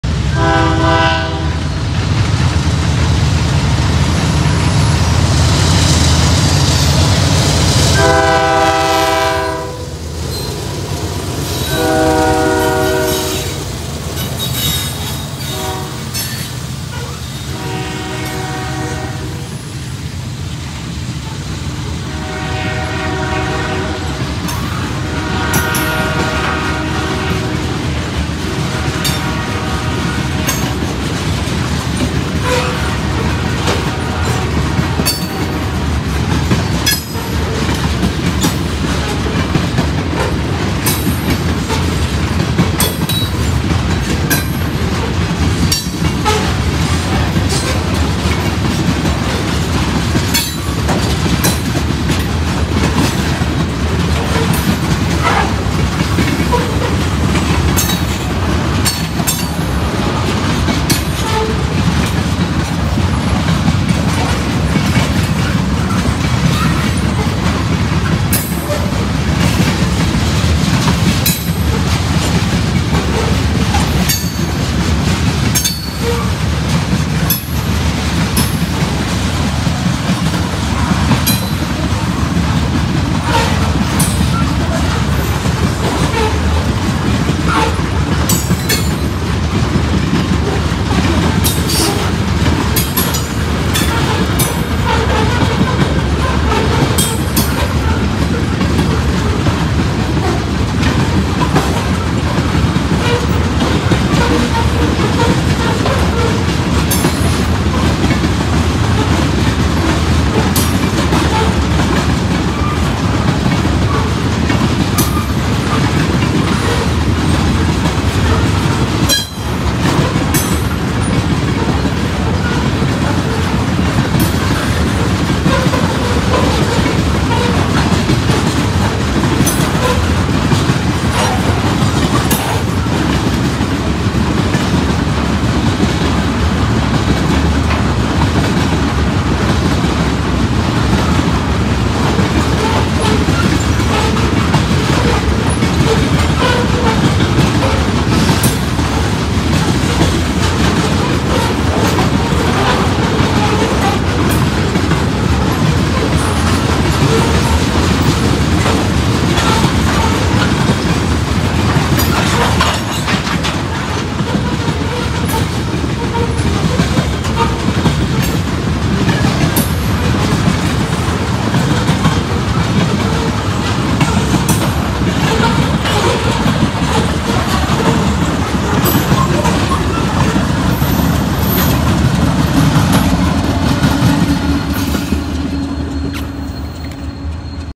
CSX B241-14 with 4 Canadian National locomotives through Cordele
Sorry for the finger at the end of the video